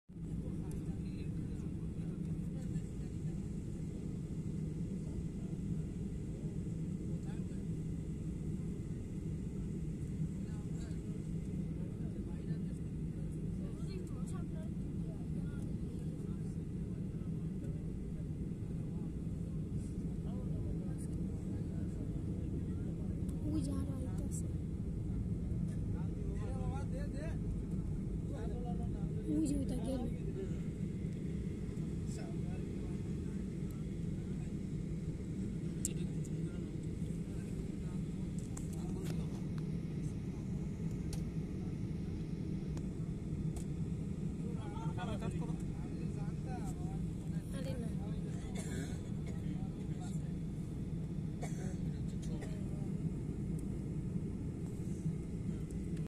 Biman Bangladesh Aircraft now ready to fly | hazrat shahjalal international airport | Biman Take Off
Ready to takeoff
Biman bangladesh
hazrat shahjalal international airport
runway
Take off
Biman
Aircraft